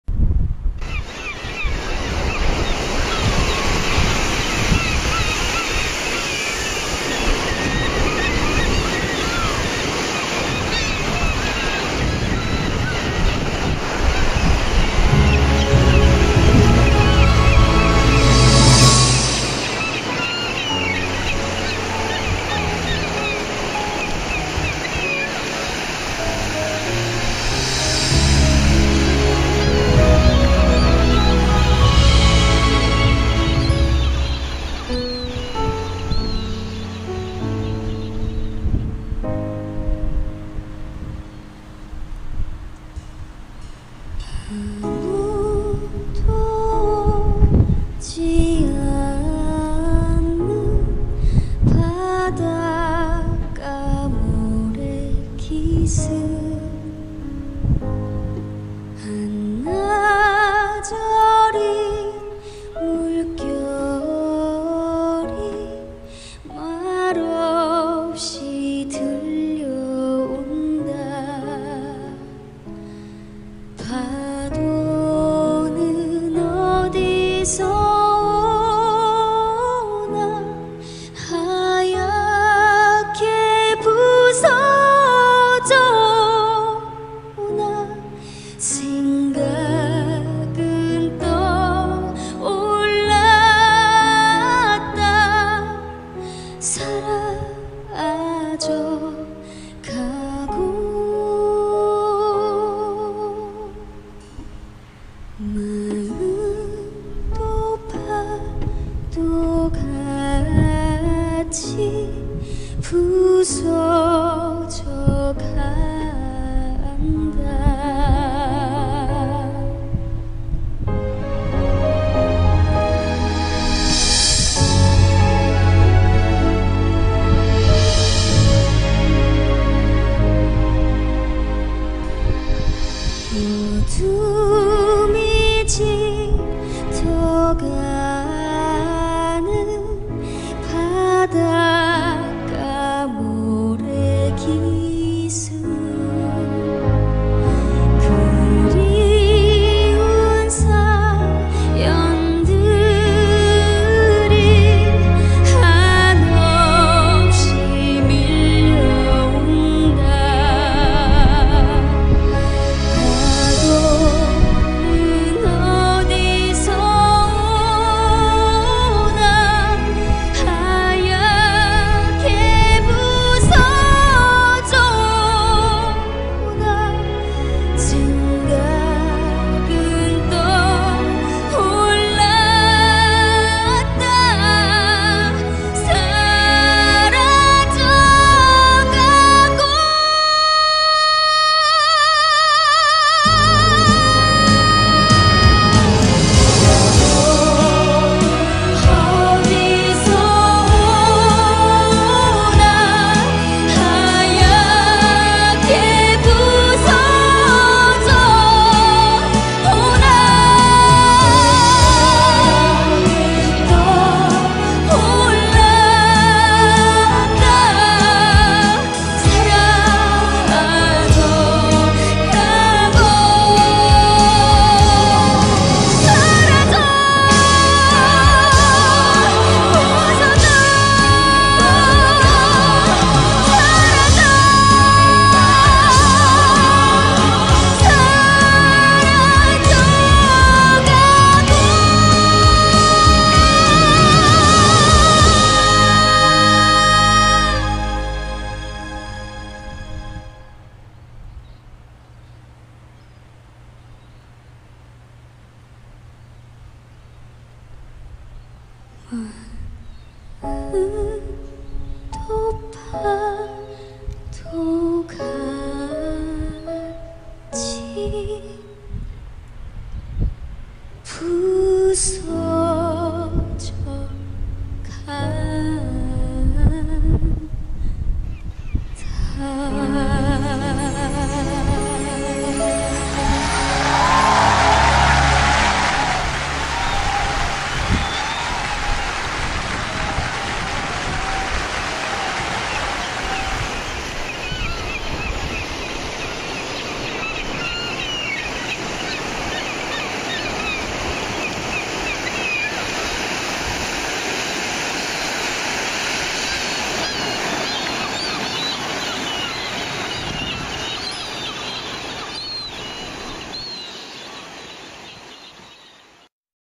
꽃지 파도 그리고 갈매기
2014년 가을을 보내며
최진사가 꽃지해변에서 파도와 갈매기를 벗삼다.